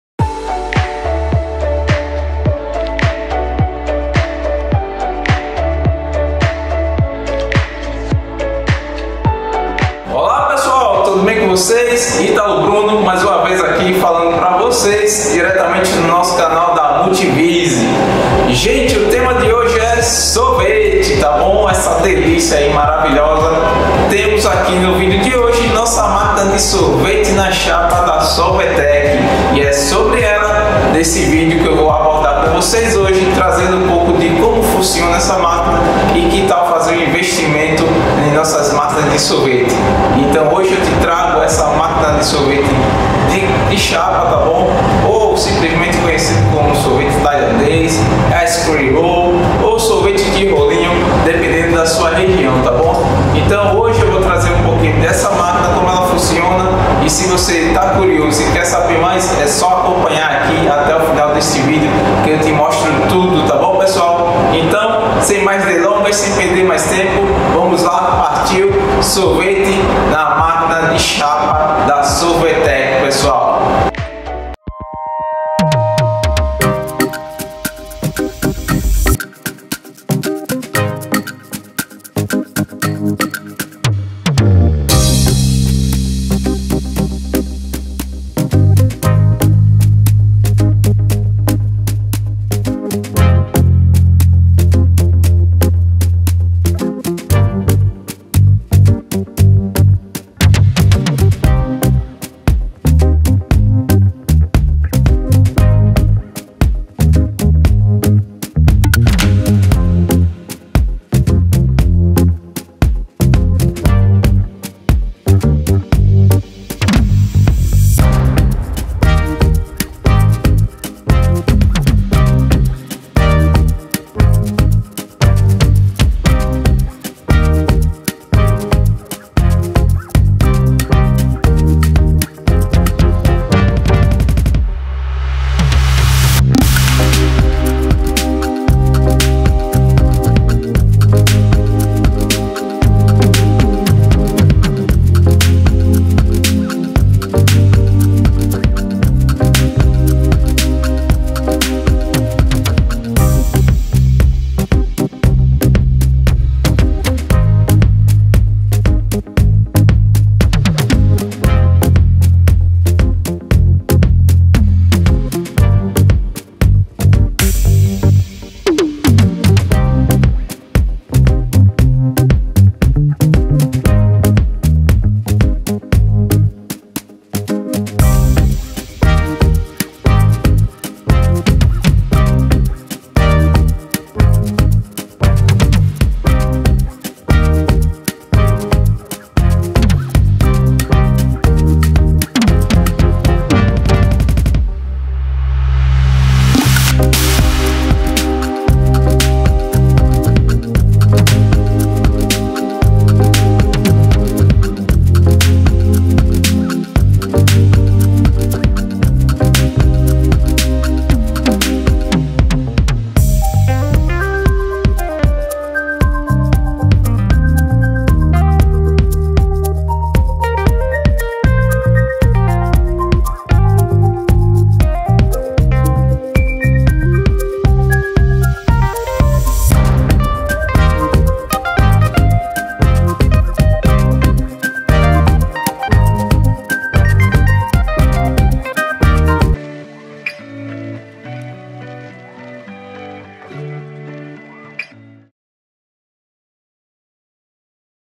Sorvete Ice Cream Roll na Máquina da Sorvetec
_________________________________________________________________________________________________________________________________________________________________________________________________________ Produza sorvetes saborosos de modo rápido e de qualidade com essa máquina de sorvete na chapa da Sorvetec, essa máquina possui 5 cubas para guardar os ingredientes e para mantê-los conservados. Nesse vídeo fizemos um delicioso sorvete na chapa com bis. quer saber mais sobre essa e outras máquinas da Sorvetec? Confira o link abaixo.